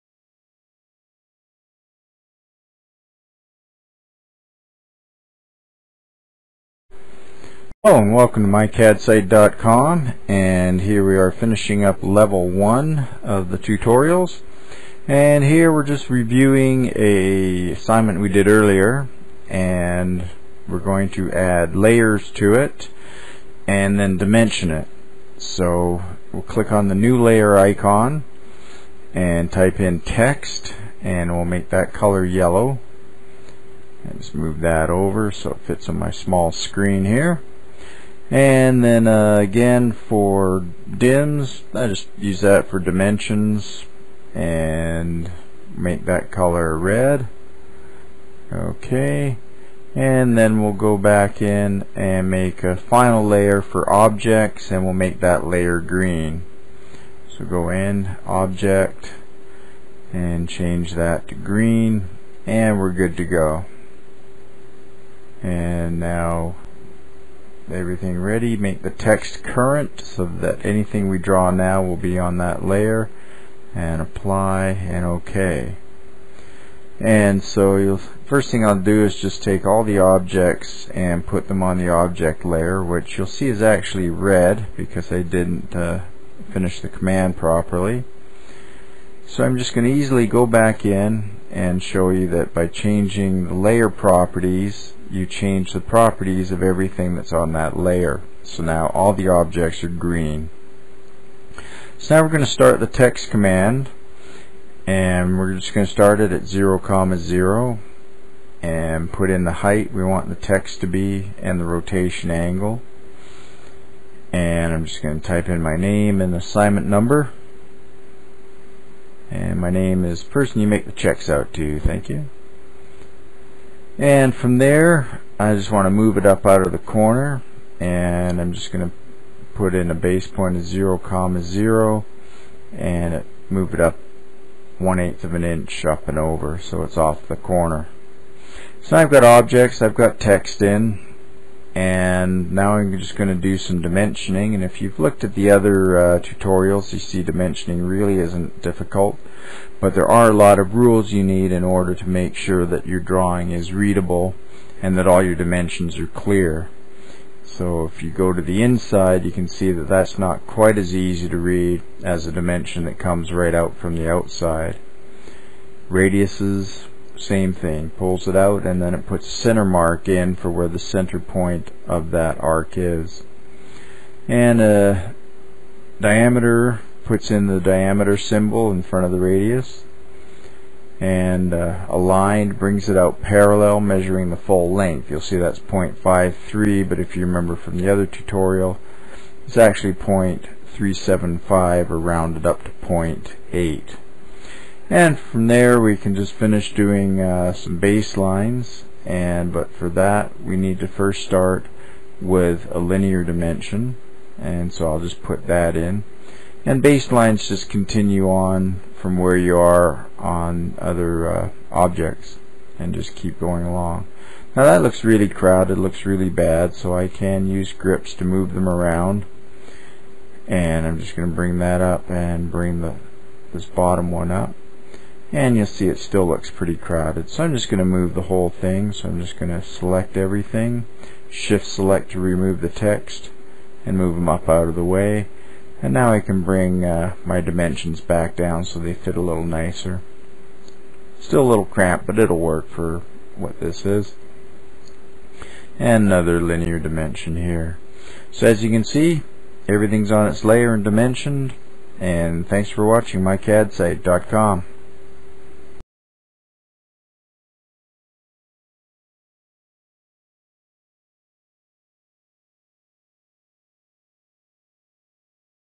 AutoCAD Tutorial Lesson 1-8 Assignment 6 (Complete Beginners Course)
Now that you've learned a lot of the drawing and modifying commands, it's time to go to the next level. This lesson will introduce you to text, dimensioning and the concept of layers. Without dimensions you can not communicate what size your objects are. Is it a real house or a doll house? Text is used to convey information that is needed other than size - materials, manufacturing process, part numbers, etc.

What you will do is take one of your previous assignments, save it to a new name and then add text and dimensions to it. These are the commands you'll be learning.